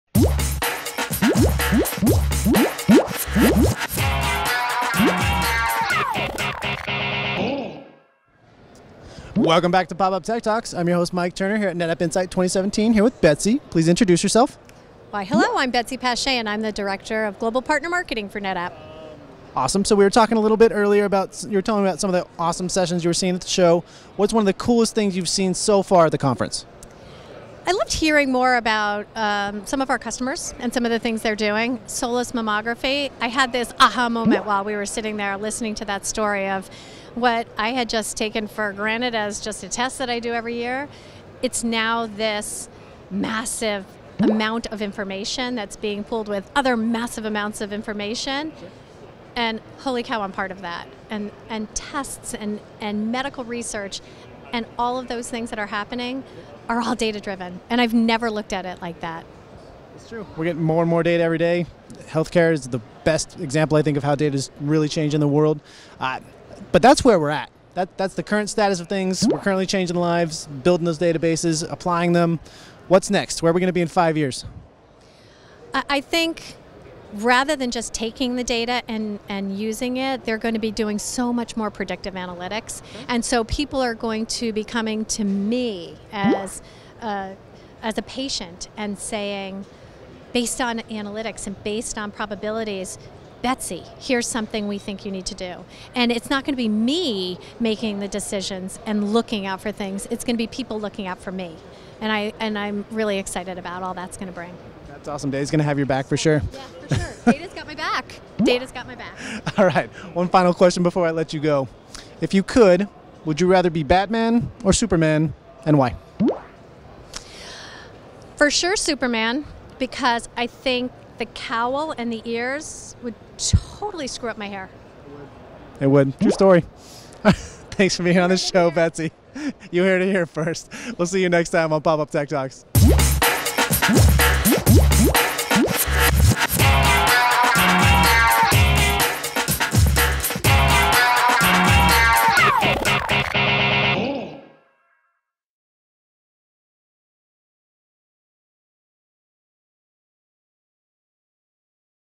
PopUpTechTalks: Betsy Pashayan @ NetApp Insight Las Vegas 2017
This time on PopUpTechTalks Betsy Pashayan shares her "Holy Cow" moment about how she is contributing to big data and how she sees the industry moving toward predictive analytics.